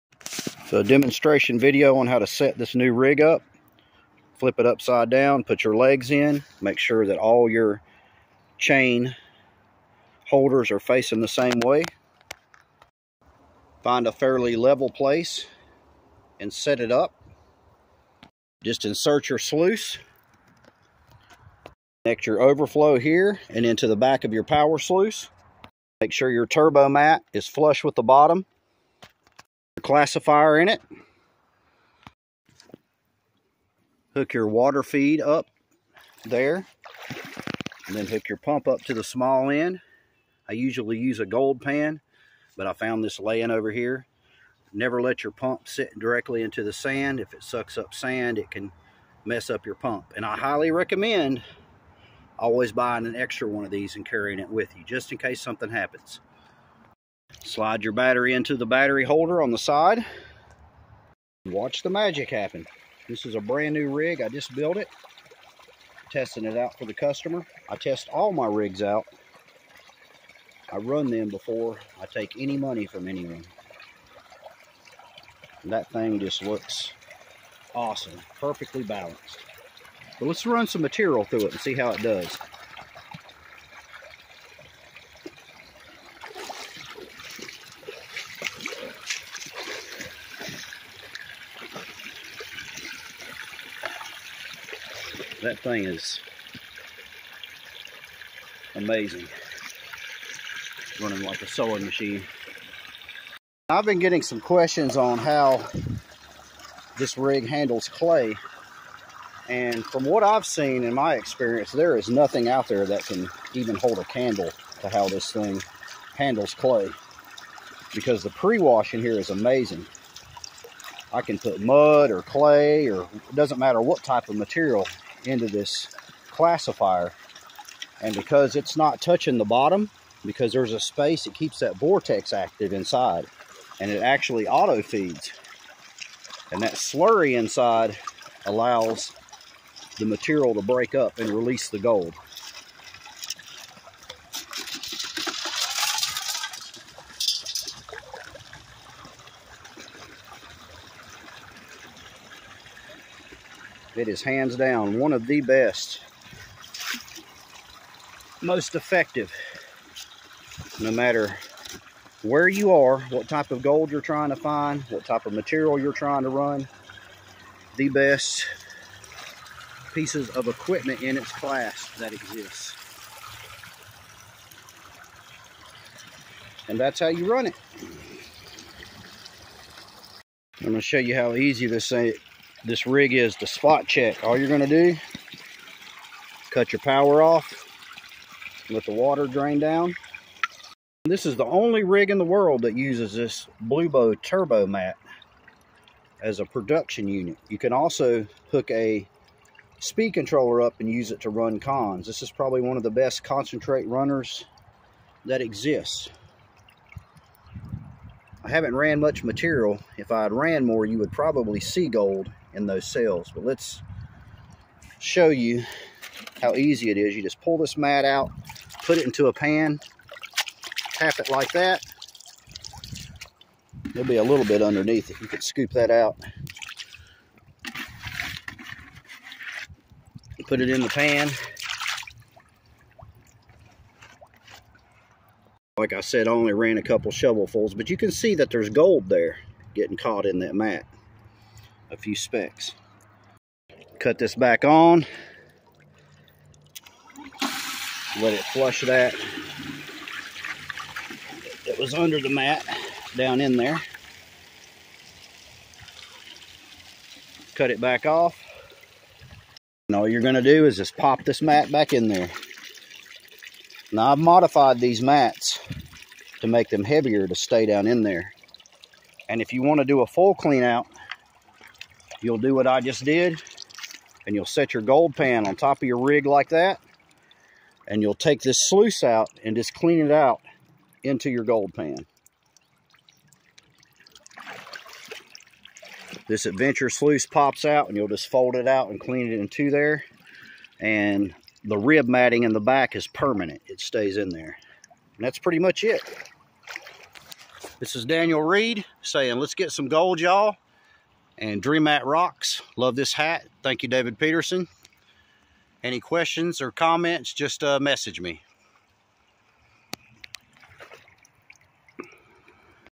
Turbo rig 🌀 instructions
how to set up and run your turbo rig from Reid's rigs. one of the most technically advanced gold mining rigs in its class in the world